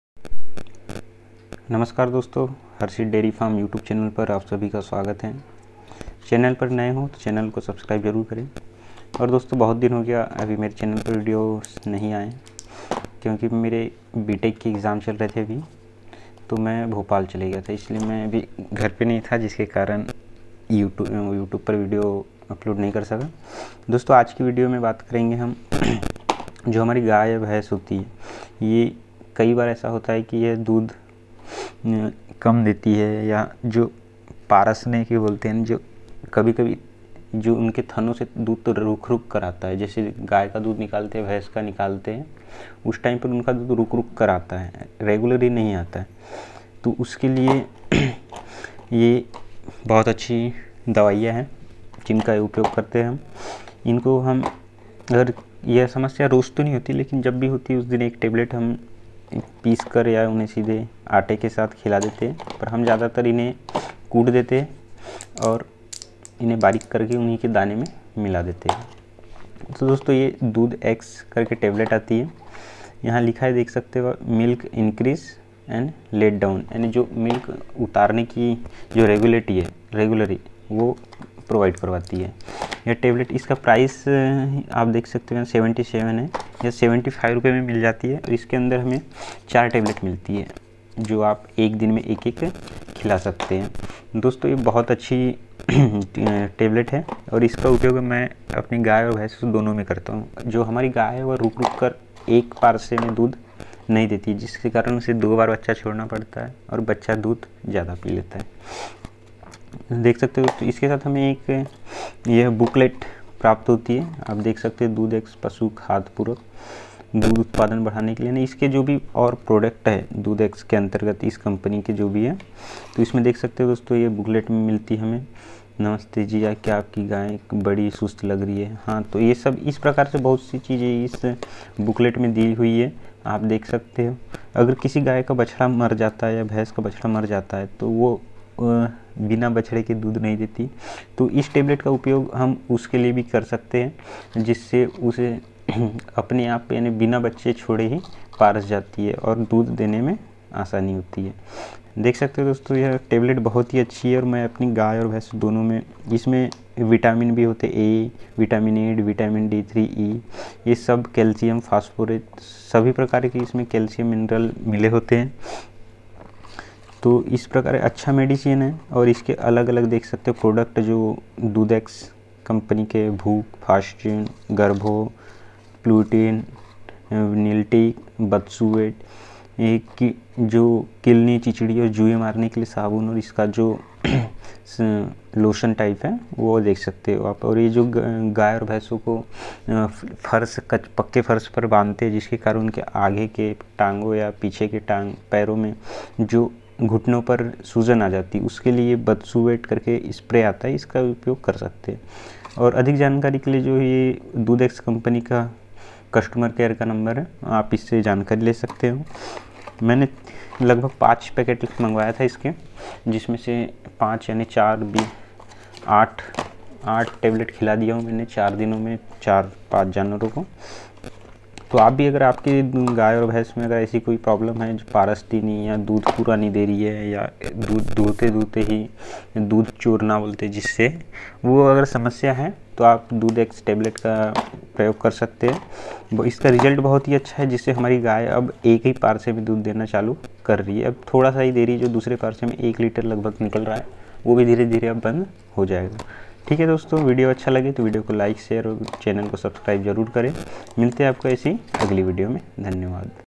गाय और भैंस का दूध उतराना // Dudh Ex Teblet // Milk Increasing & Letdown
Agar aapki gay aur Bhains dudh Puri tarike se nahi utaar rhi hy ya Paras Nahi rahi hai to aap dudh ex tablet ka prayog kar sakte hain ya tablet bahut hi acchi hai jisse aapki Sabhi Prakar ki samasyaen khatm ho jayegi ismein bahut se khel se mineral hai jo gay Aur bhainson Ke Liye bahut upyogi hai.